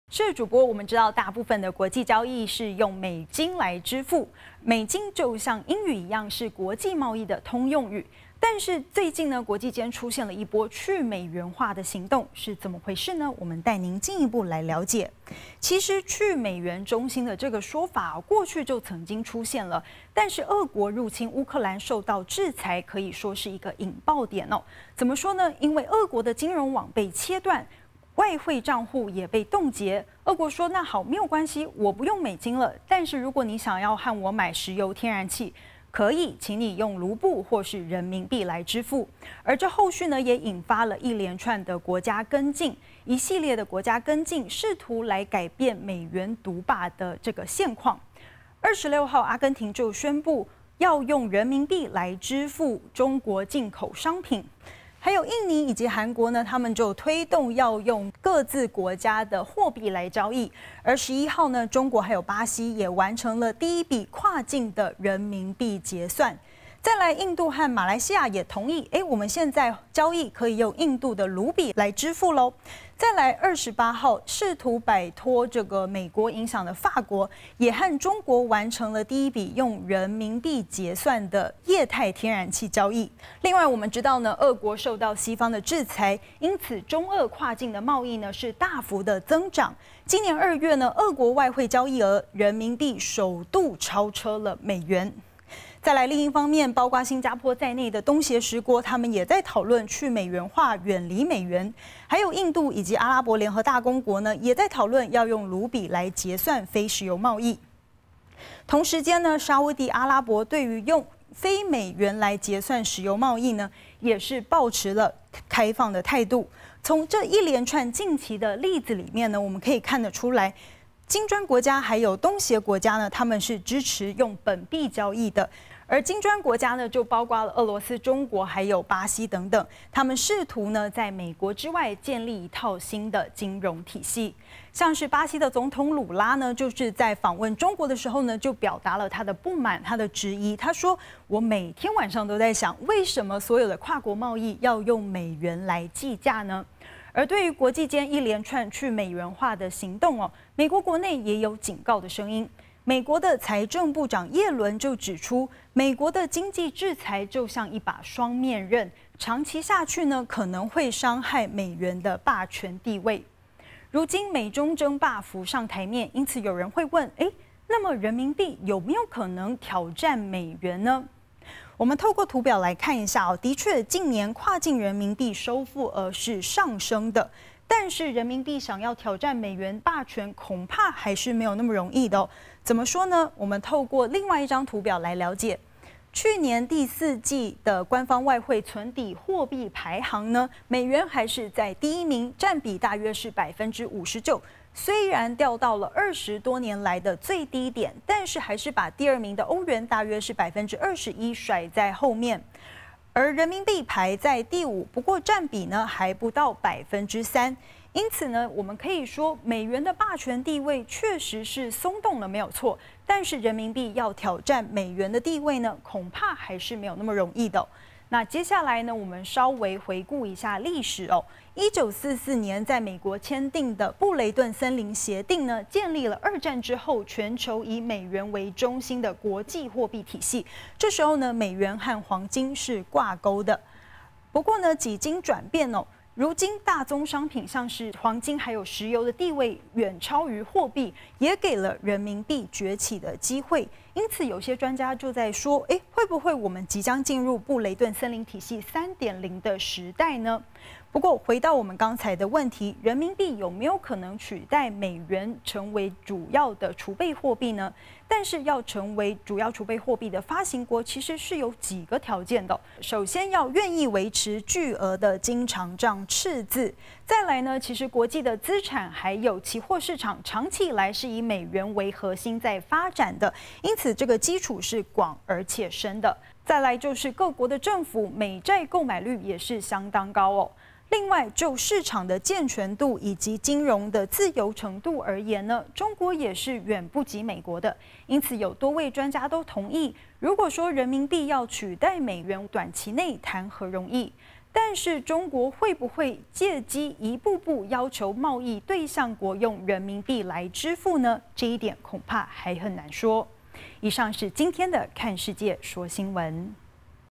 益起看世界-俄遭國際制裁 改用盧布及人民幣貿易 倡議「去美元化」 金磚五國和東協多國 推本國貨幣交易 美元霸權遭動搖嗎?專家說資本市場健全 金融相對自由 取代美元不容易
俄國入侵烏克蘭遭美國制裁，開始不用美元，改用盧布和人民幣進行貿易結算，金磚五國和東協成員國迅速跟進，改用自己國家的貨幣交易，甚至研擬採行新貨幣。

中國和法國更在3月底，完成首次以人民幣結算液態天然氣交易。
一連串的行動，威脅美元霸權地位，人民幣有可能取而代之，成為主要國際貿易貨幣嗎？
請國際新聞中心編譯許齡文，為您解說。